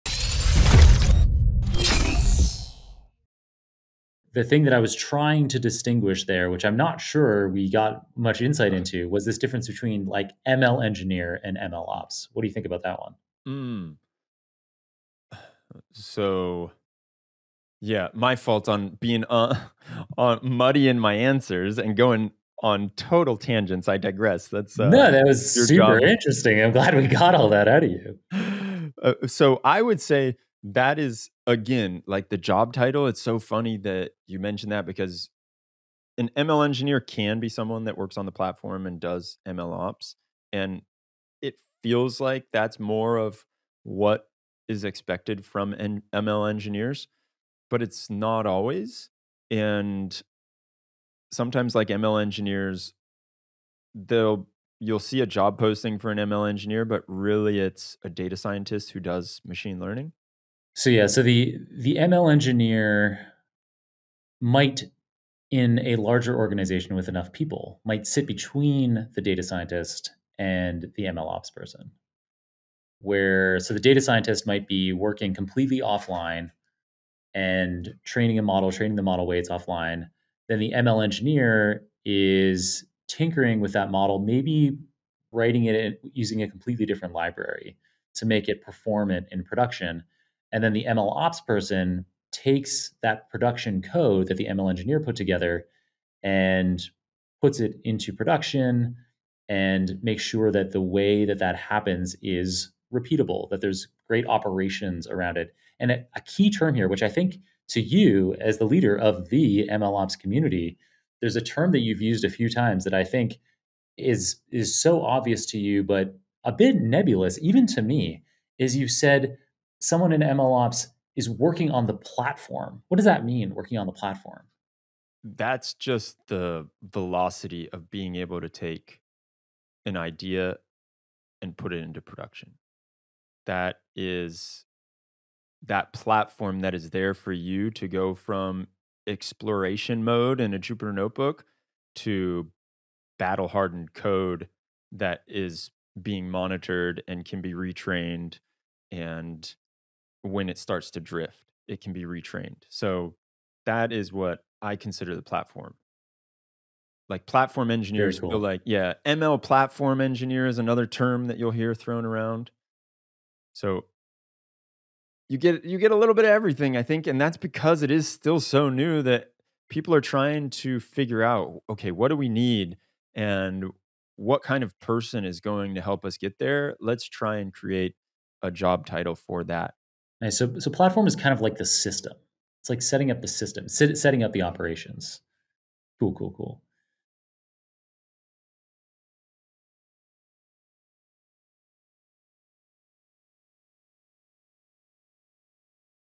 MLOps vs ML Engineering Explained
Learn about MLOps, how to build an online community, and tools for scaling LLMs as Demetrios Brinkmann joins @JonKrohnLearns to speak about the similarities and differences between LLMOps, MLOps and DevOps, and why this should matter to companies looking to hire such engineers. You will also hear how to get involved in the MLOps community (@MLOps) wherever you are in the world, and how you can start developing great products with the available tools.